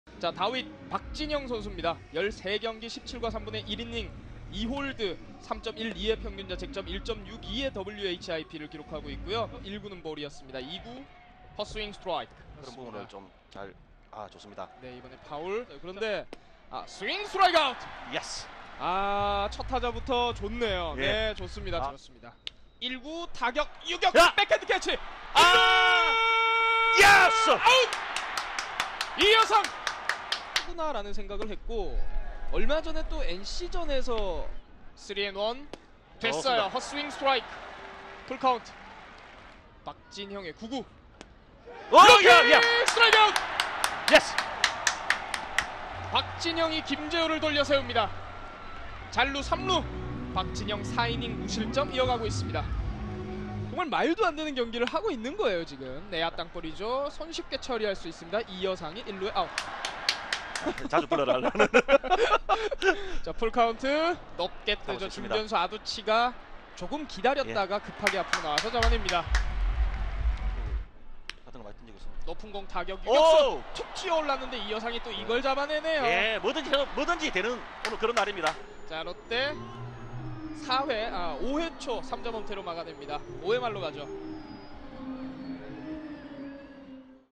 [한줄요약] 5월 22일 일요일 vs 두산 - 데뷔 첫 승
데뷔 첫 선발등판, 데뷔 첫 승.

[5월 22일 일요일 對 두산베어스]

- 두산베어스 4 : 10 롯데자이언츠
- 승리투수 : 박진형 (5이닝 2피안타 무실점)
- 결승타 : 이여상 (4회, 적시 2루타)
- 4회, 김문호-최준석 연속타자 홈런